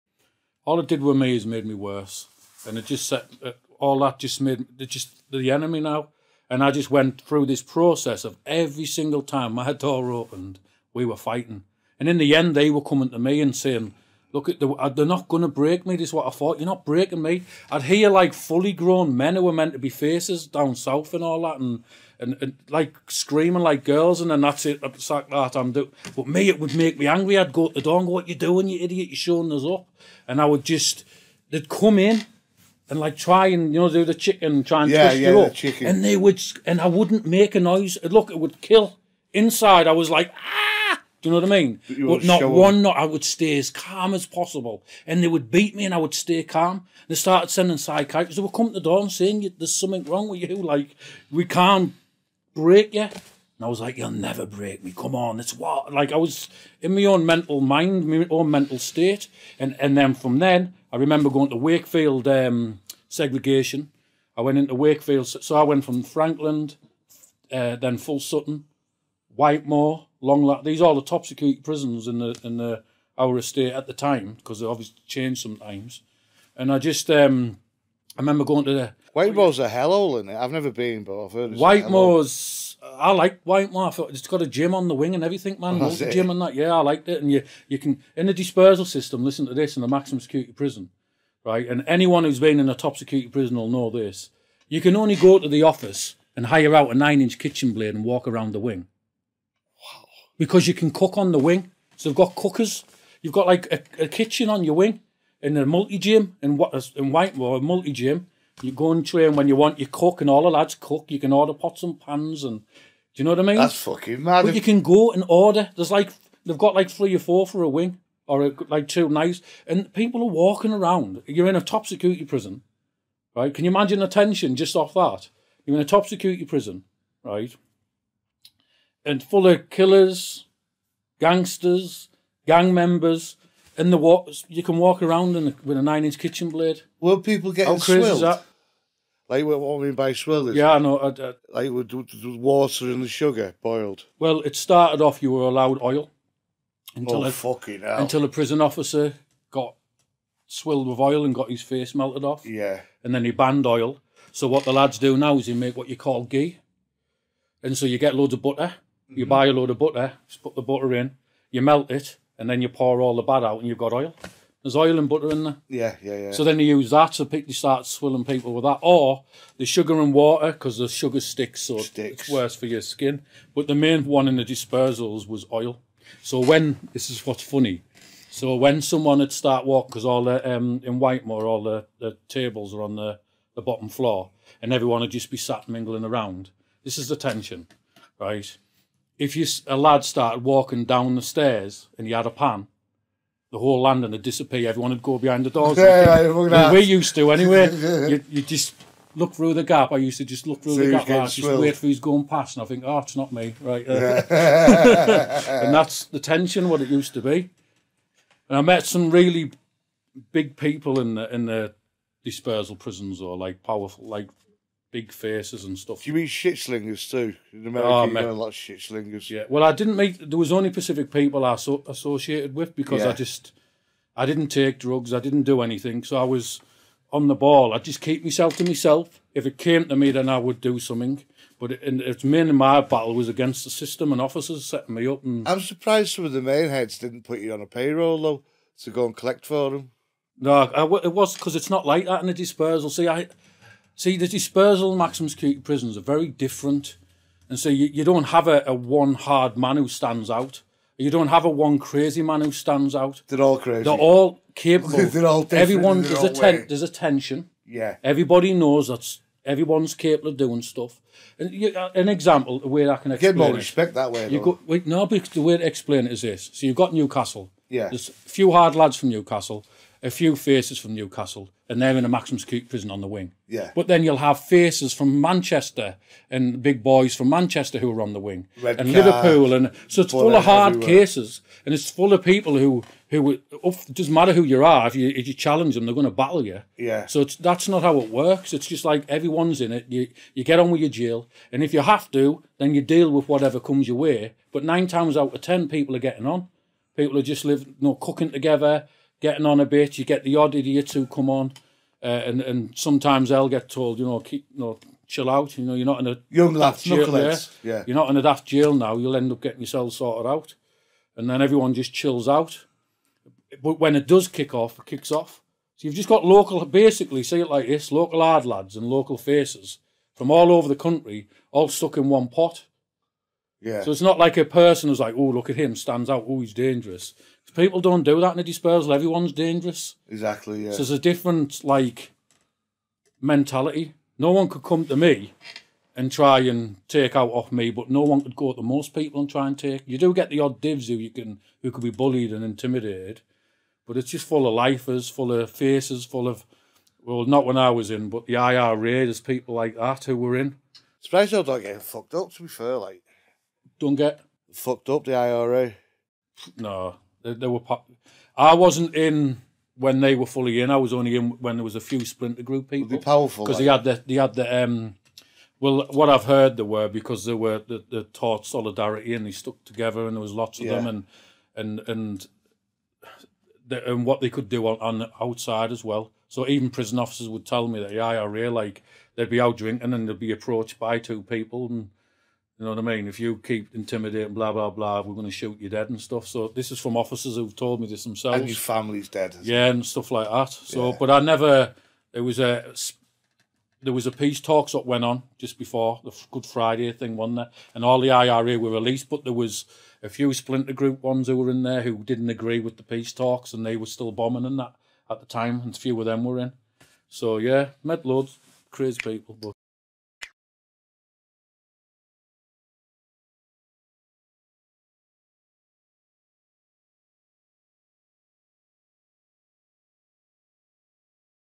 Shane Taylor v UK Supermax Prison Guards: with Wild Man
Classified as one of the most dangerous prisoners in the UK, Shane Taylor went on a 17-year rampage of attempted murder, theft and drugs until a dramatic encounter forced him to search his soul.